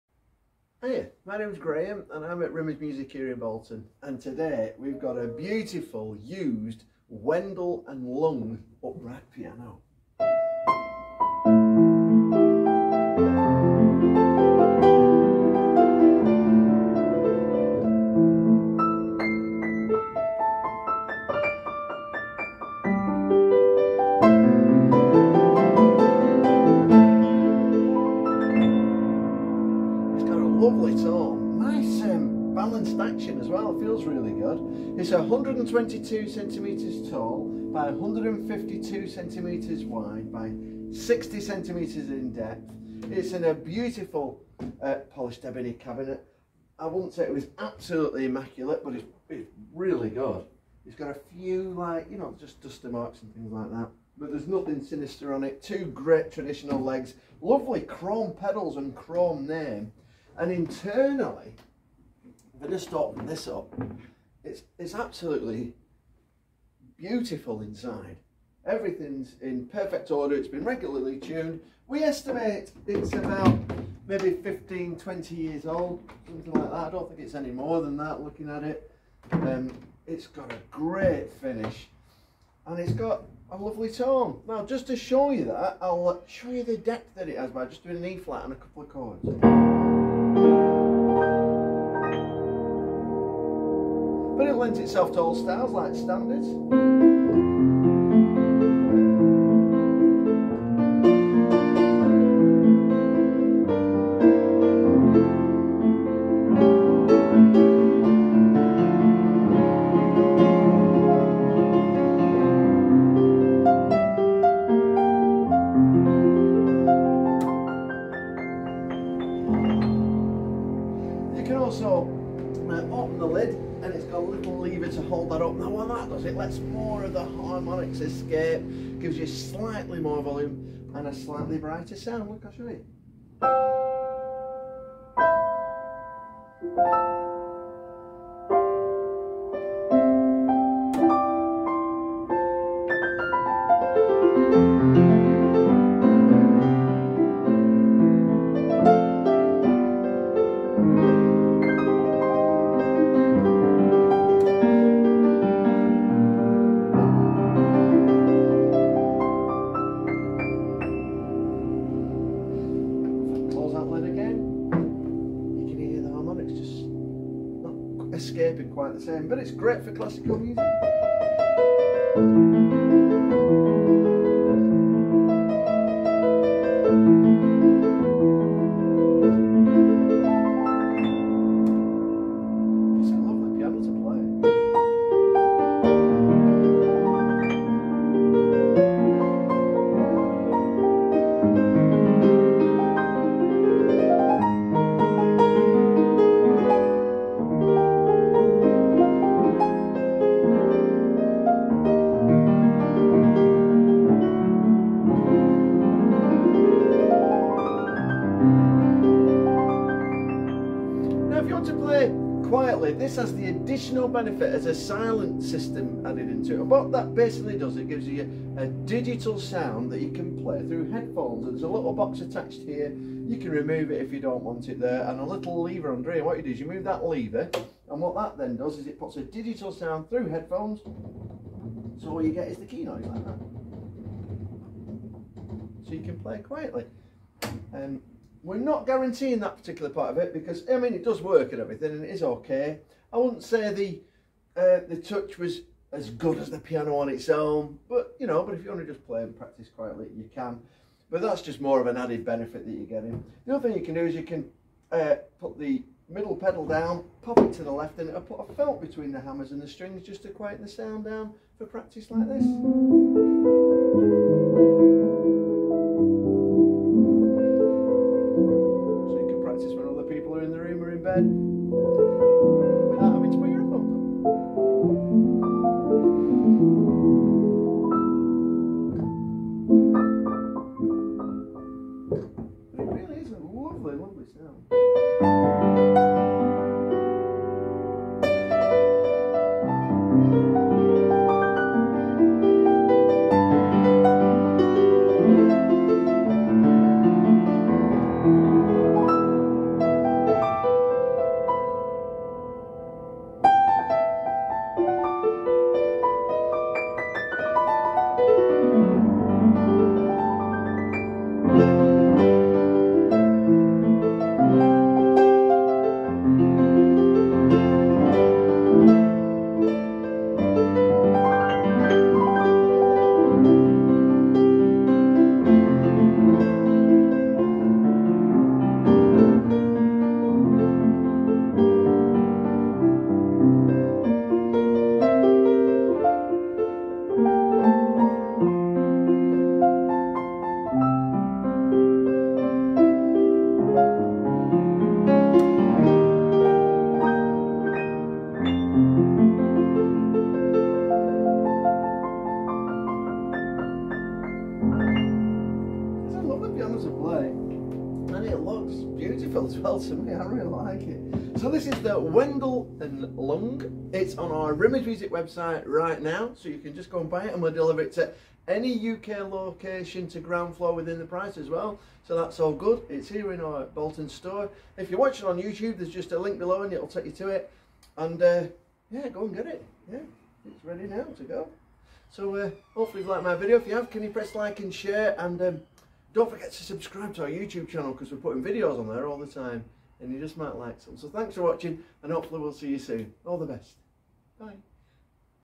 Used Wendl & Lung 122 Upright Acoustic Piano Demonstration & Review | Rimmers Music
Graham Blackledge demonstrates & reviews a Used Wendl & Lung Model 122 Silent Upright Acoustic Piano at our Rimmers Music Bolton Store.

Free Delivery To Anywhere In UK Mainland (Ground Floor With Easy Access)
Tuned & Prepared Before Delivery
3 Month Guarantee (Piano Only And Not The Silent System)

Used Wendl & Lung Model 122 Silent Upright Acoustic Piano | Bolton Store | Serial No. WL50369

At Rimmers Music we are very proud to present for sale this stunning Used Wendl & Lung Model 122 Silent Upright Acoustic Piano in the finish of Polished Ebony. The condition of this piano is good, it does have a few cosmetic marls on the cabinet and polishing scratches on the top but these are purely cosmetic. The tone of this piano is lovely and it has a really nice key feel and response. This piano comes fitted with a silent system by Wendl & Lung which is currently working but may need calibrating, we are not guaranteeing the system. The price we are selling this piano is reflected on it being a stunning acoustic piano, the silent system as merely an added bonus. The piano has been tuned and prepared by our very own piano tuner/technician and we are including free delivery to a ground floor easy access location anywhere in UK mainland.
James - Rimmers Music.